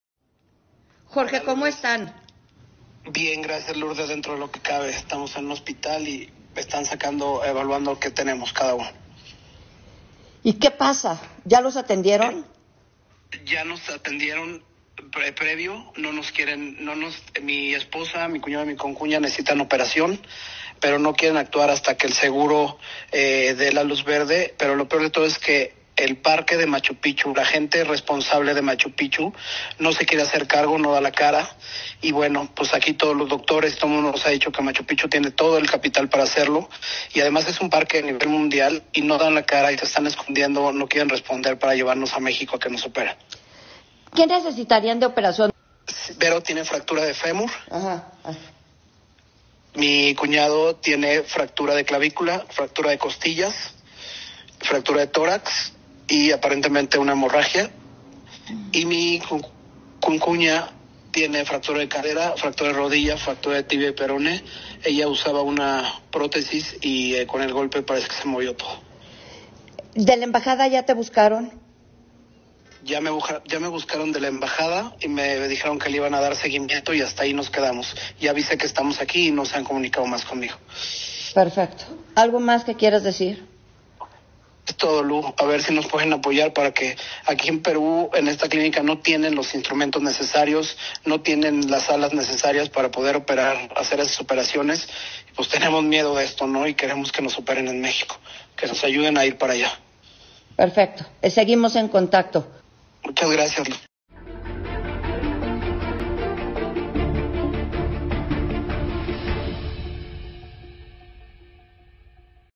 3 mexicanos necesitan operación tras accidente en Machu Picchu; acusan al parque
Un autobús que transportaba a turistas extranjeros, incluidos mexicanos, sufrió un grave accidente este lunes al volcarse en una carretera cercana a la famosa ciudadela inca de Machu Picchu. El incidente dejó un saldo de cuatro heridos con fracturas múltiples, según informó la Secretaría de Relaciones Exteriores (SRE) de México.